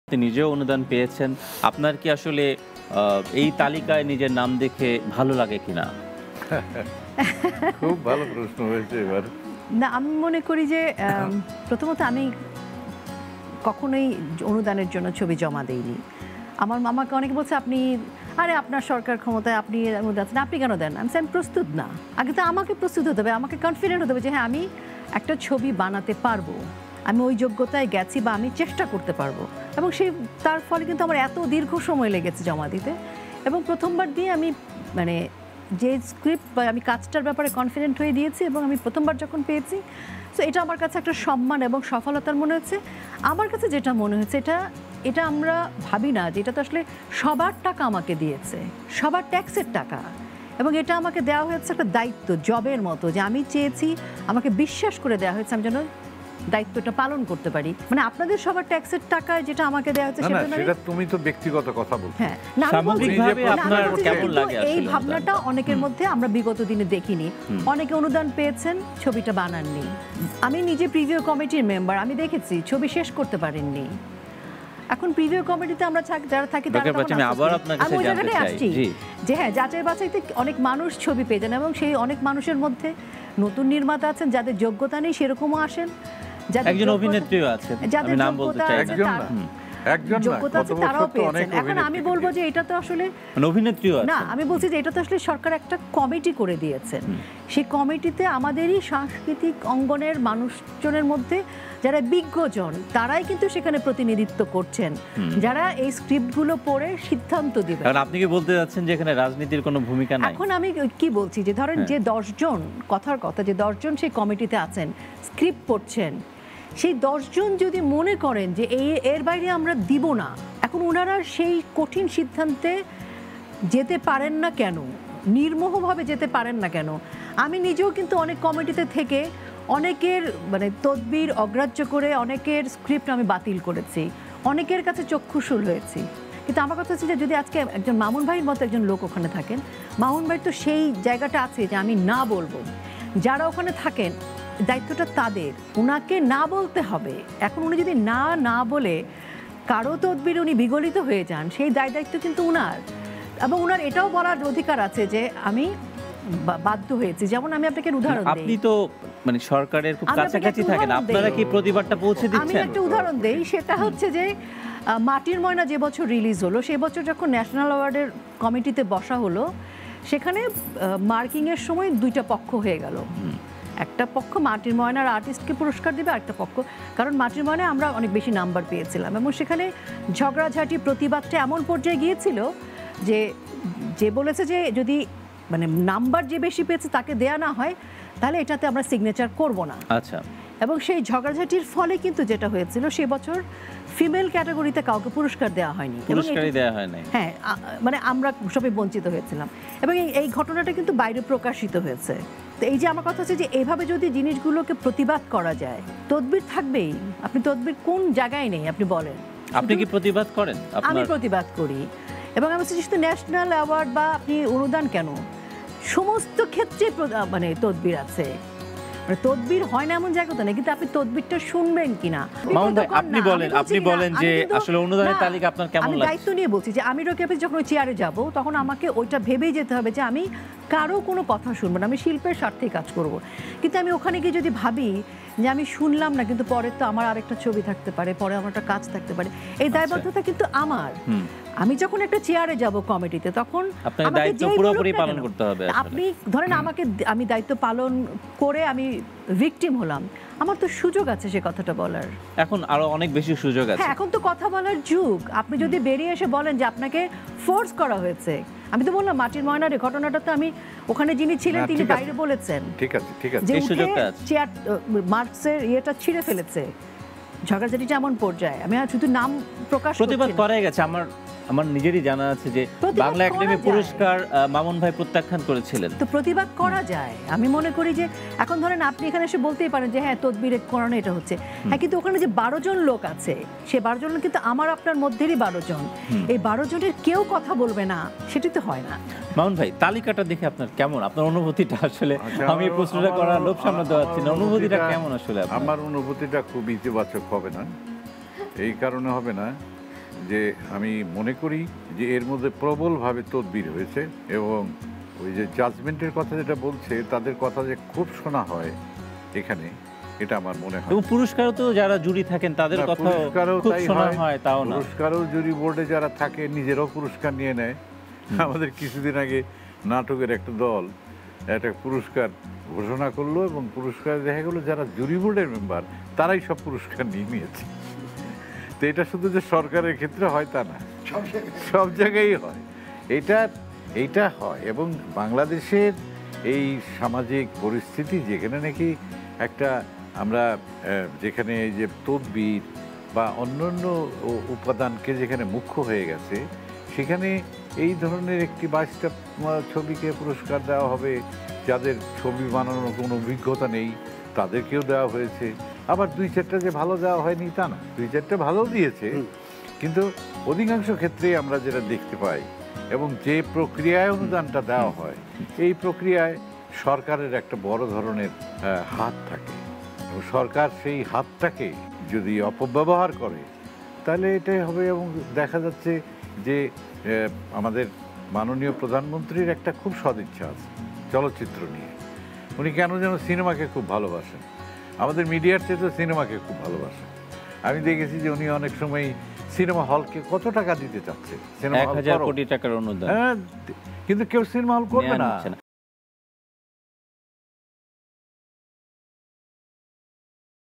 সরকারি অনুদান যারা পান তাদের বেশিরভাগই ‘অনভিজ্ঞ’ - বললেন রোকেয়া প্রাচী | Rokeya Prachy | Nagorik TV
সরকারি অনুদান যারা পান তাদের বেশিরভাগই ‘অনভিজ্ঞ’ - বললেন রোকেয়া প্রাচী